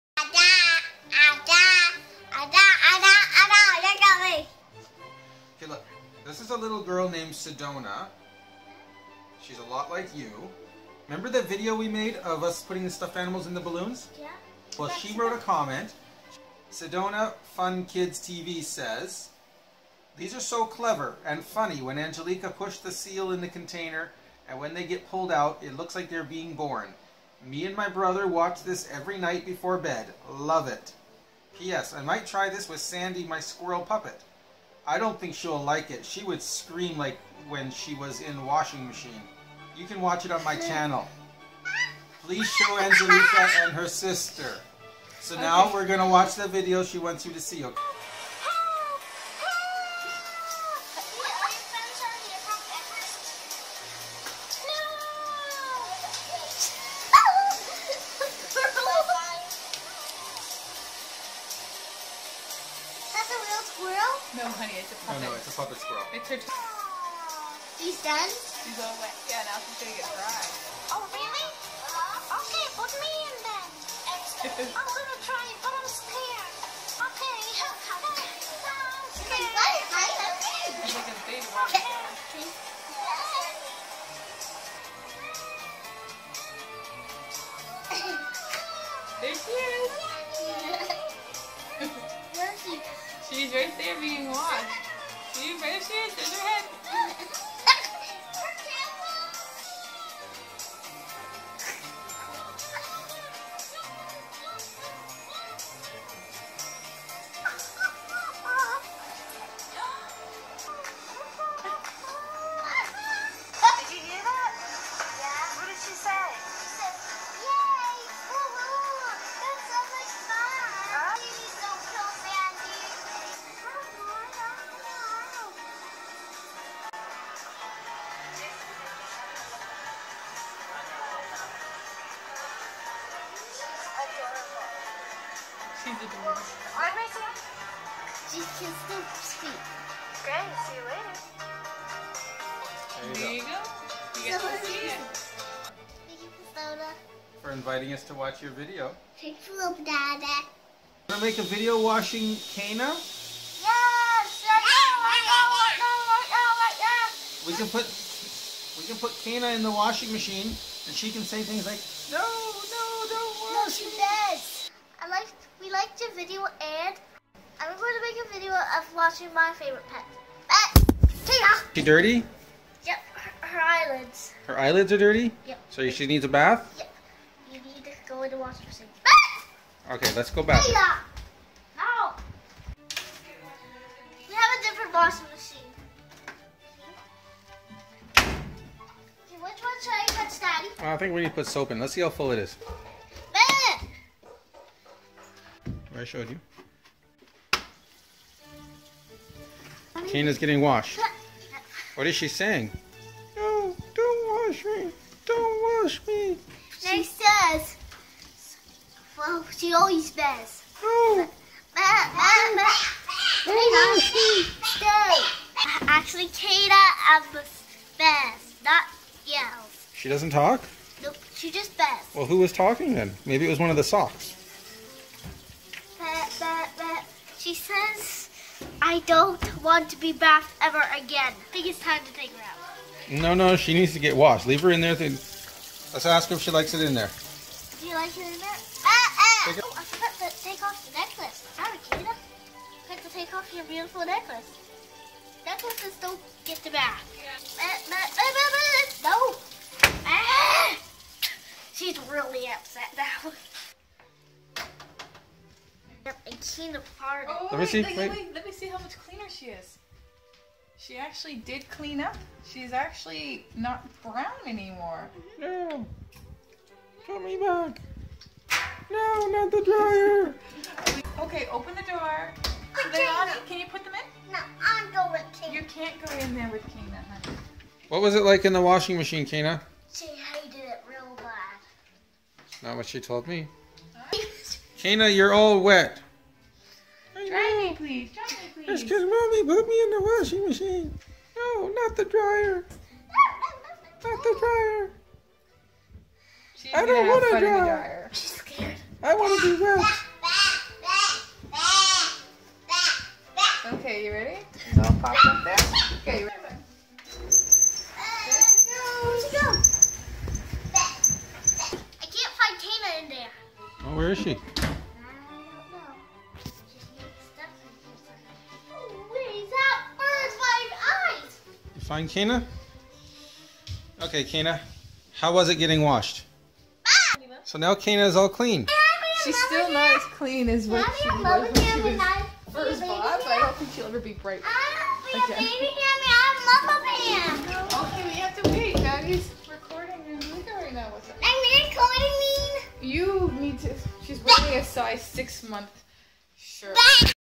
Bathing her favorite stuffy | video response for Sedona Fun Kids TV, before they were famous
I got a lovely comment on my "How to put a stuffed animal in a balloon" video

who asked that I show one of her videos to my girls.  We then decided to make our own video giving one of our favorite stuffies a much needed bath.

We appreciate all the positive comments and feedback our channel has been getting.

josh_bruni